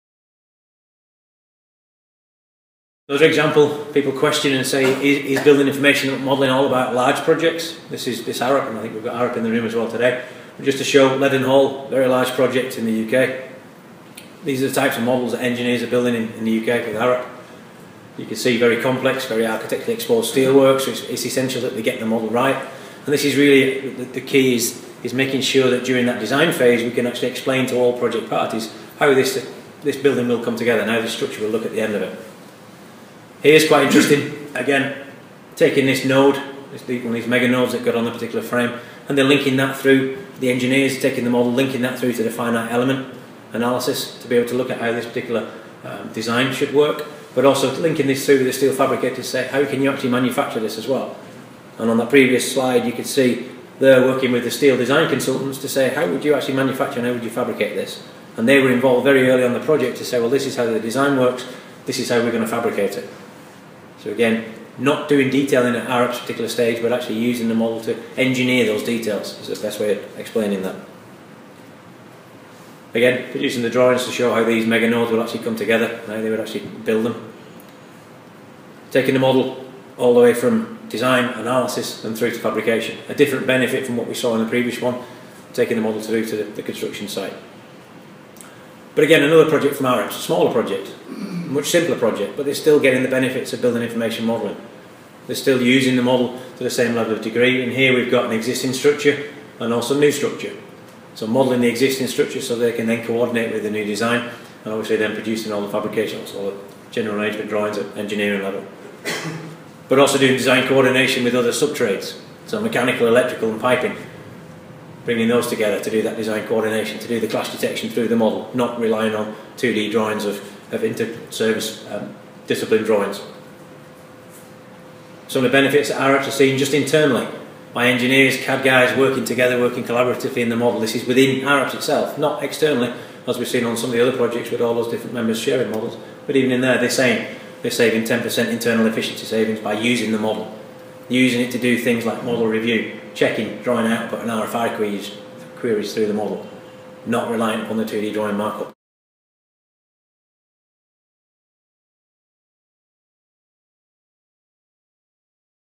BIM in Action: Arup UK Case Study
See two projects from Arup in the UK, and how they use BIM to enhance their efficiencies. (2.35 min)